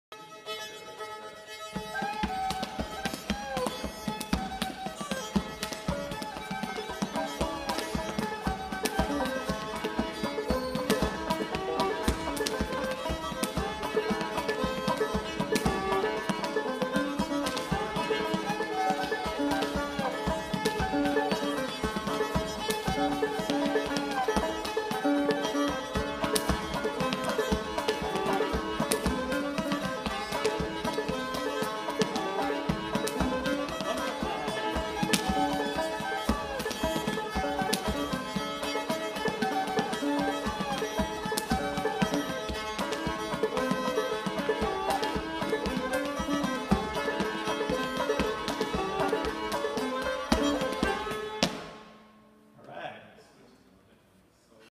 Hamilton College Folk Musicians Perform
Professor Lydia Hamessley, Jake Blount '17 and Phil Jamison '76 perform “Ithaca style” of music at the Swannanoa Gathering in North Carolina.

This is Hamilton's promise to students: Study what interests you, be accepted for who you are, and prepare to be the person you were meant to become.